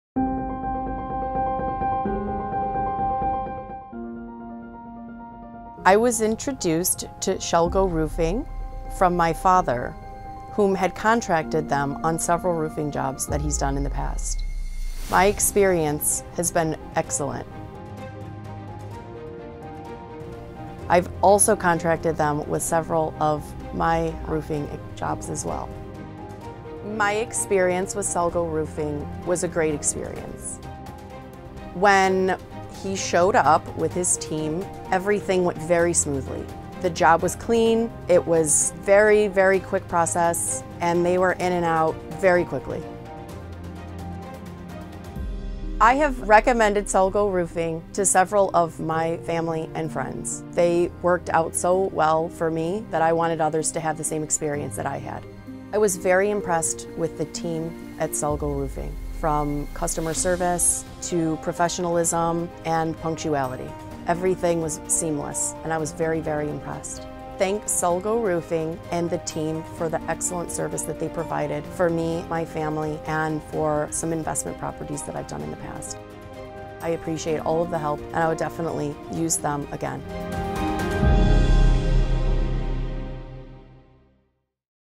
Salgau Roofing Testimonial Lindsey Erdmann - Full Version
Salgau Roofing, Toledo OH. About us.

Our customer speaking about his experience with Salgau Roofing
Always Professional, that's a promise!